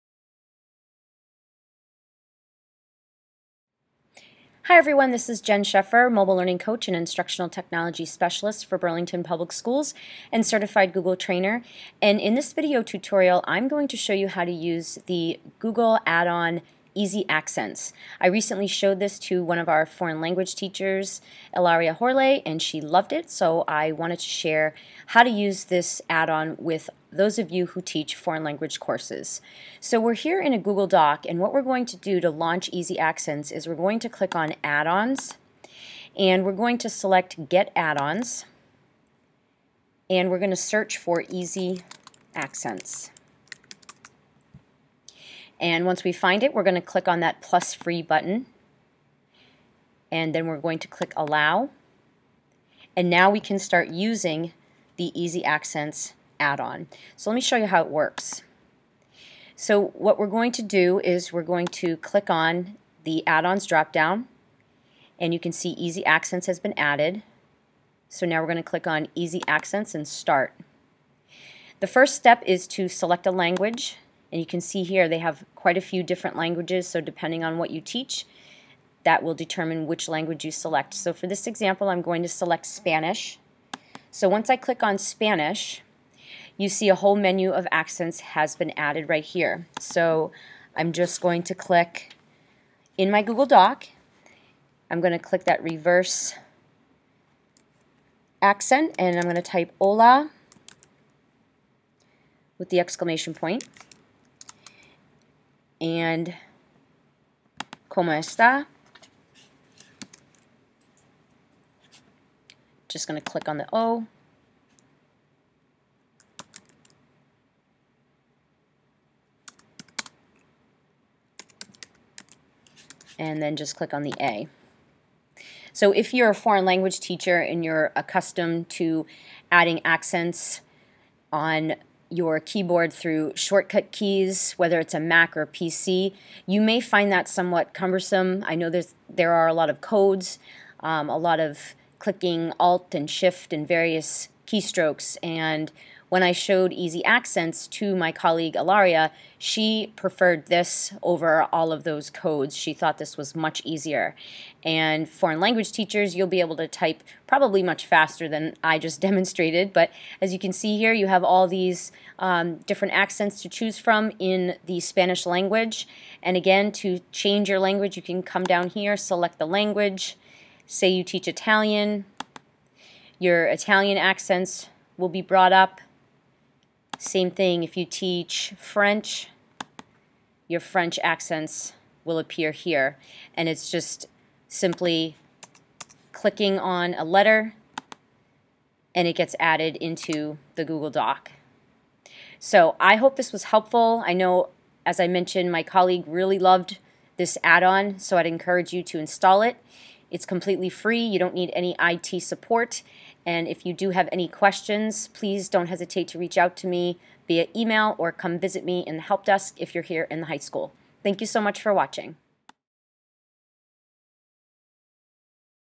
Easy Accents in Google Docs
A walk through of how to use Easy Accents in Google Docs